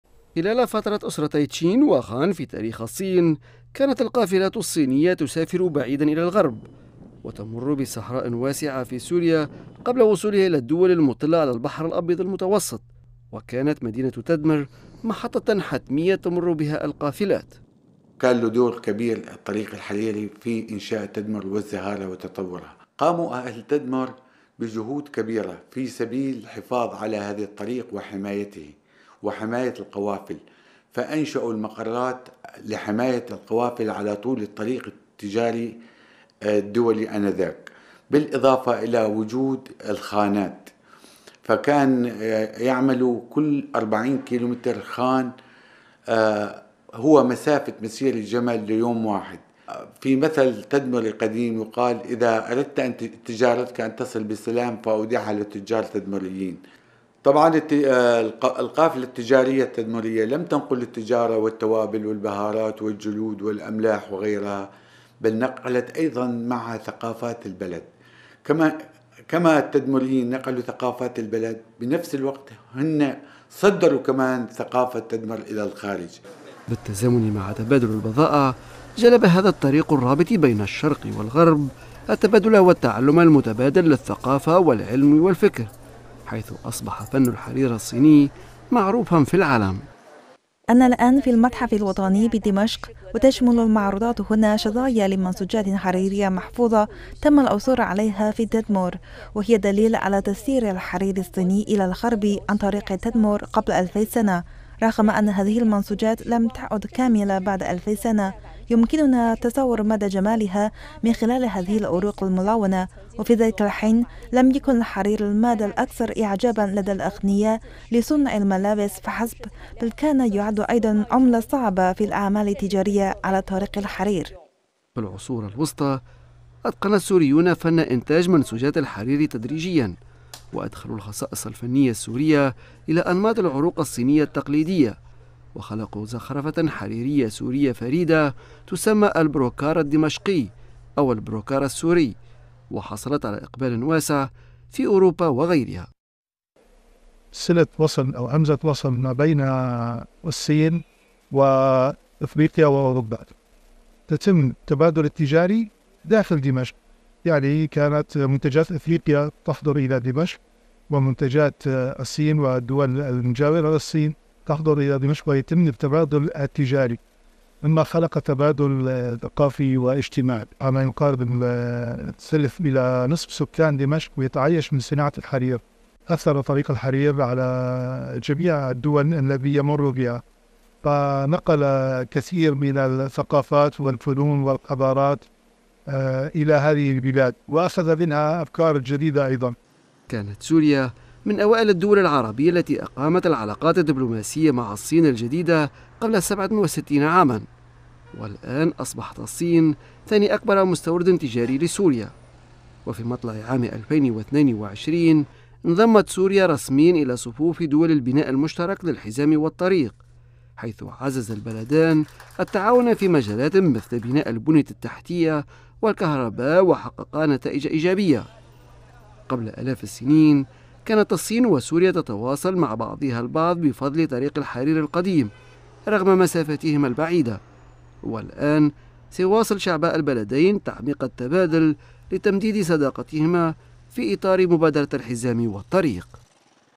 مدينة تدمر القديمة: أسطورة على طريق الحرير
أدرجت مدينة تدمر القديمة في قائمة التراث الثفاقي العالمي لليونسكو، وتعرضت لتخريب خطير بسبب احتلال التنظيمات المتطرفة لها مرتين. لكن قبل أكثر من ألفي سنة، كانت المدينة مركزا تجاريا مزدهرا يربط بين الخليج العربي ودول شرق آسيا وبين البحر الأبيض المتوسط والدول الغربية. وبمناسبة الذكرى العاشرة لطرح مبادرة الحزام والطريق، دعونا نتعرف معا على تغيرات هذه المدينة وتطوراتها بفضل طريق الحرير.
الاشتراك في فيديوهات قناة من خلال اليوتيوب